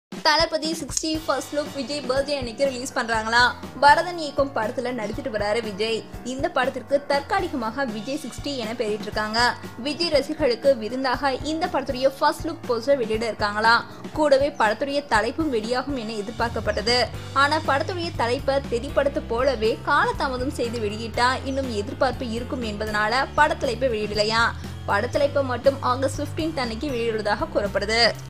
Vijay 60 shooting spot - Vijay candid clicks from thalapathy 60
Vijay 60 - Jagapathi Babu plays baddie in Vijay 60. Vijay's upcoming film will be shot in Tirunelveli district in Tamil Nadu soon. Major part of the film is set here and the actor is learning the local dialect for his role.